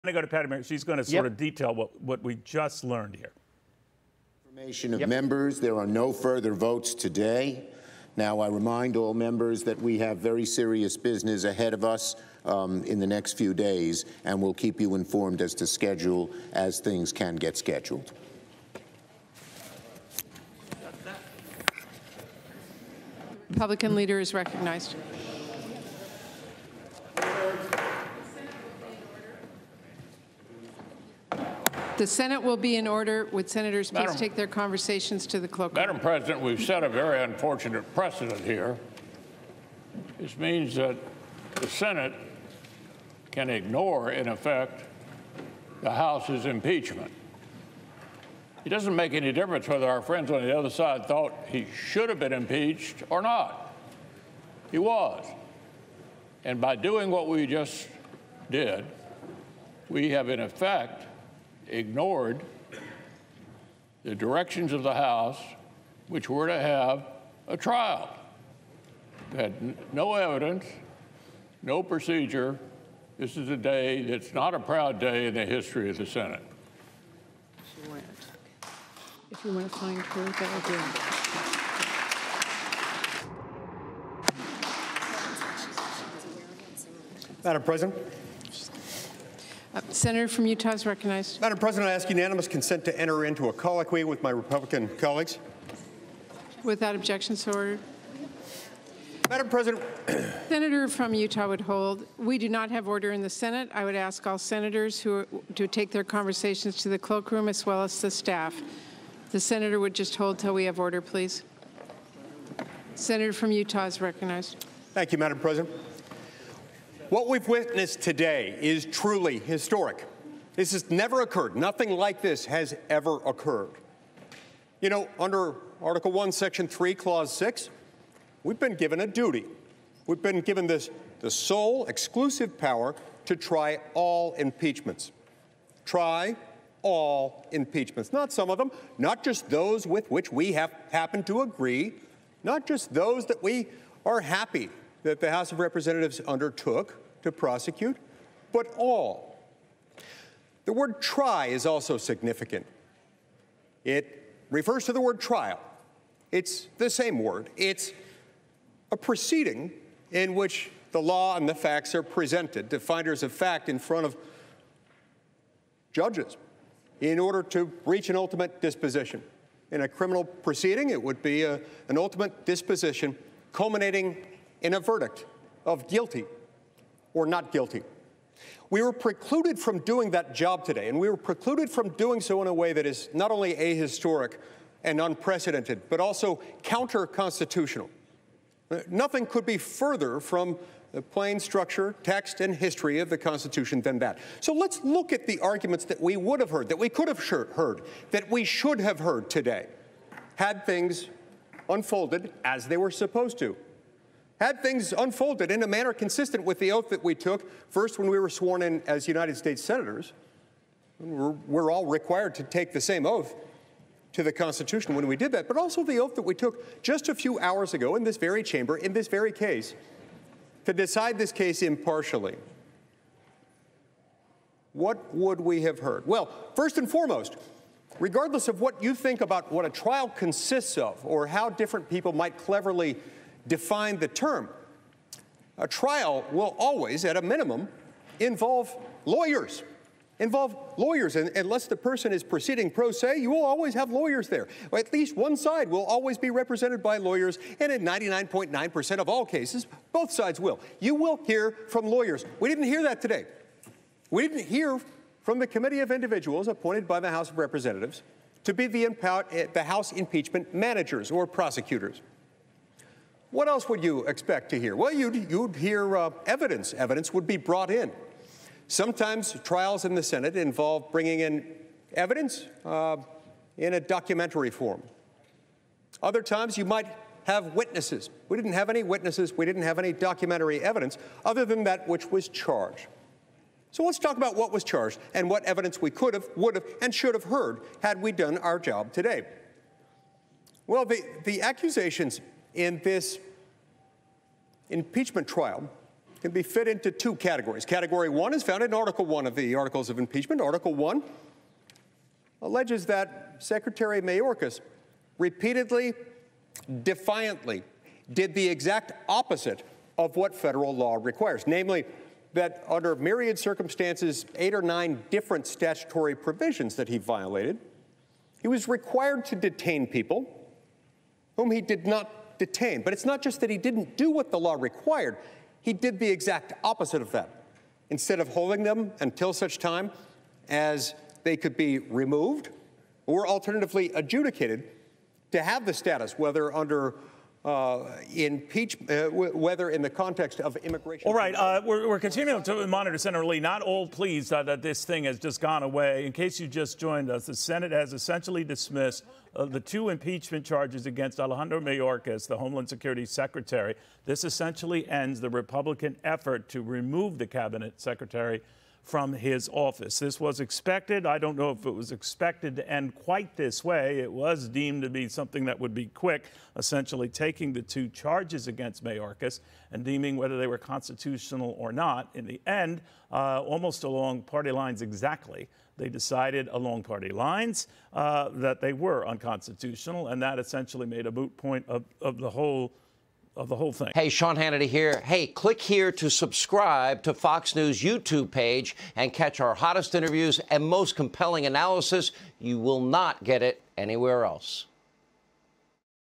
DISMISSED: Senate strikes down impeachment articles against Biden border chief
The U.S. Senate dismissed both articles of impeachment against Department of Homeland Security Secretary Alejandro Mayorkas after they were deemed unconstitutional in a party-line vote.